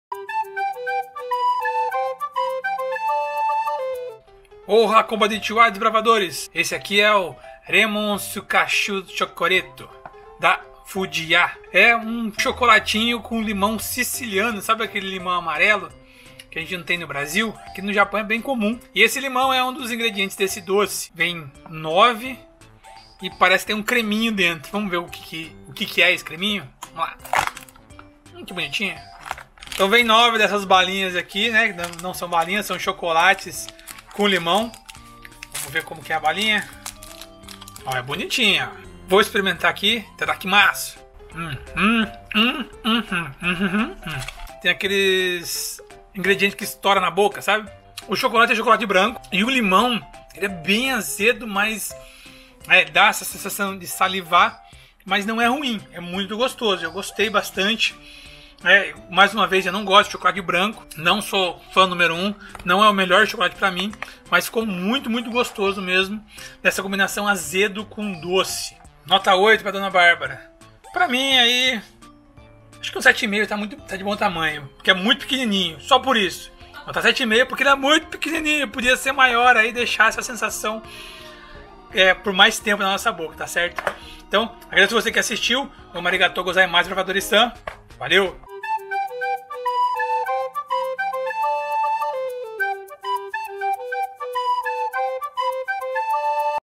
Chocolate branco com Limão (Remon Sukaashu Chocoreeto) - Itadakimassu
Esse pequeno doce reúne um delicioso chocolate branco ao sabor azedo do limão siciliano, com uma pequena surpresa que estoura na boca.